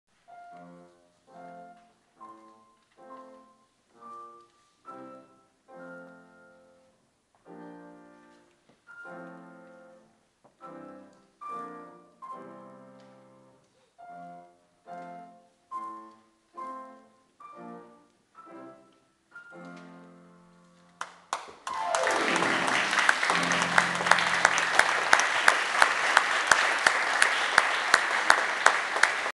Recital - Piano, Mount Pleasant, SC - Spring 2012
Short piano performance, student / teacher duet.
East Cooper Music - Mt. Pleasant SC